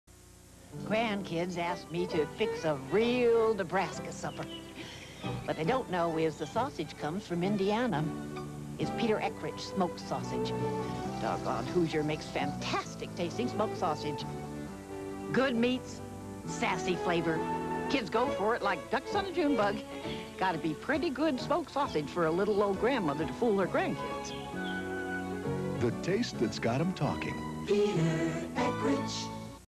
1985 Peter Eckrich Sausage TV Commercial
Grandma knows best. The commercial was originally aired on 1/24/1985 It was run during the syndicated broadcast of "Barney Miller" on KPHO Channel 5 in Phoenix AZ. KPHO Channel 5 was an independent station back then.